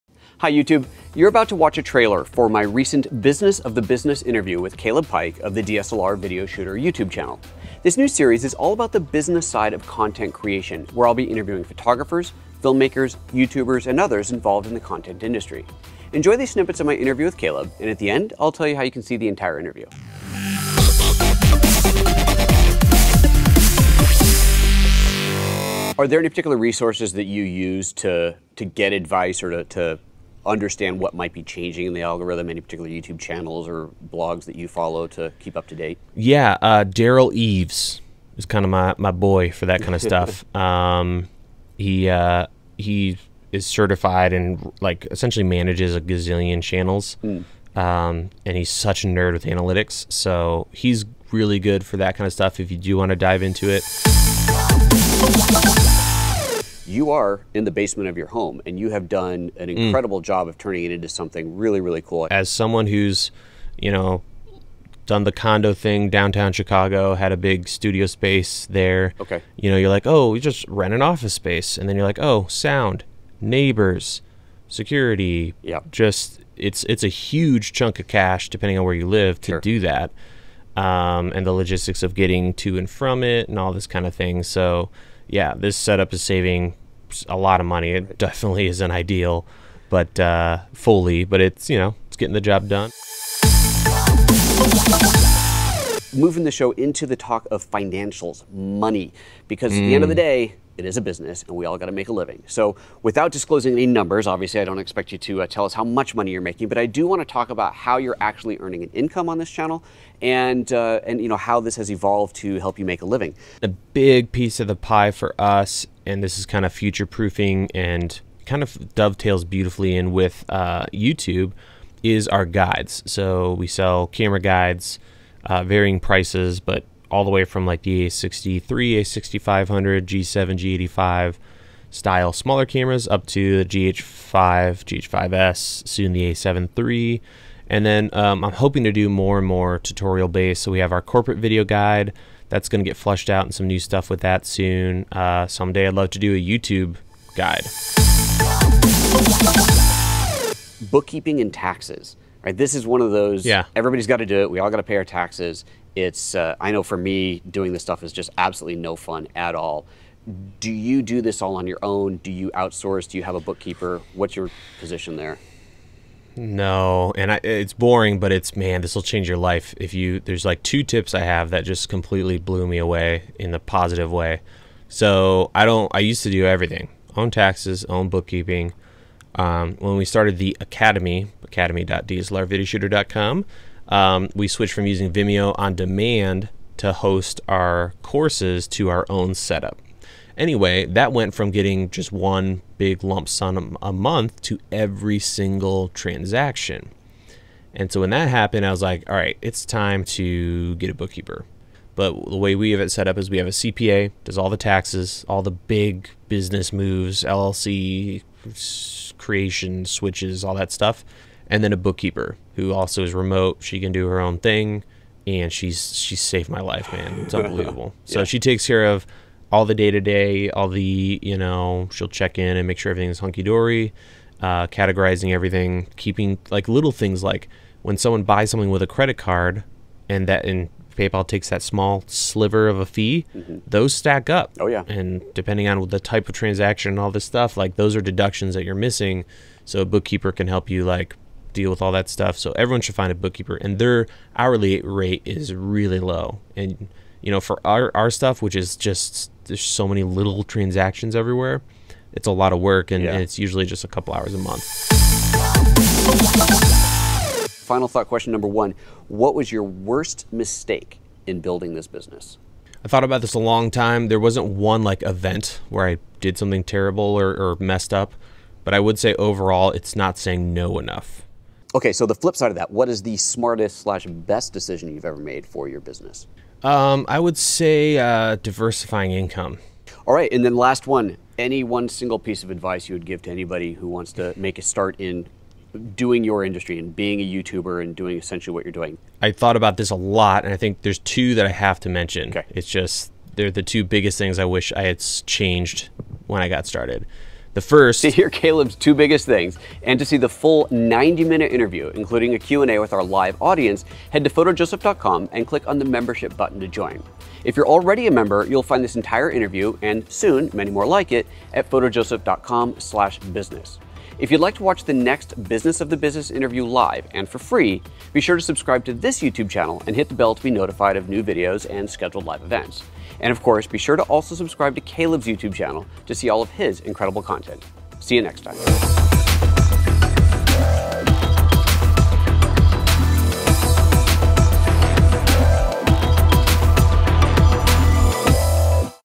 TRAILER: Caleb Pike "Business of the Business" Interview with PhotoJoseph
This is a TRAILER of the inaugural interview for the new "Business of the Business" series, talking with people in the photo, film and content creation industry about the BUSINESS side of their business. How did they get here, how do they maintain, and how do they operate behind the scenes? Our first guest is Caleb Pike of DSLR Video Shooter

The full video includes Caleb's background, how he got the channel started, and what it took to become a full time YouTuber. Then we get into financials; how he makes money, bookkeeping and accounting, and "final thoughts" answering the questions "worst mistake ever", "best decision ever", and "advice to someone just starting out". The full 90 minute interview concludes with a live audience Q&A,

To watch the full 90 minute interview, (this was the first interview, so if you go there and you only see this one, more will come soon!)